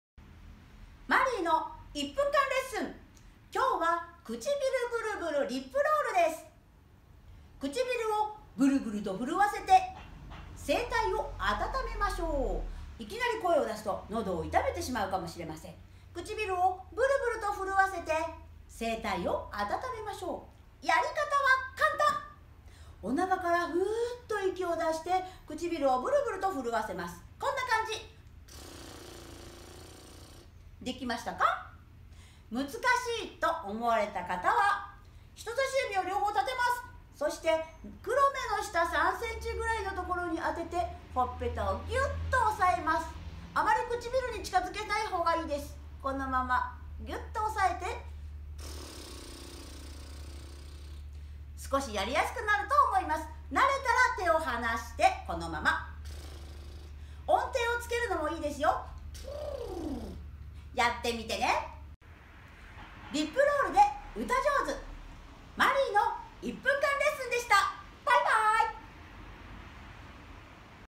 まりーの1分間Lesson 2.リップロール
はっせー＆まりーのLet’s Relax!! まりーの1分間レッスン
毎週火曜日、に配信していた「はっせー＆まりーのLet’s Relax!!」からのスピンオフ！
毎回1分間のレッスンで、どんどん歌上手になります。
動画を見ながら、実際に動いてみてください。
今回は、「リップロール」で、歌上手！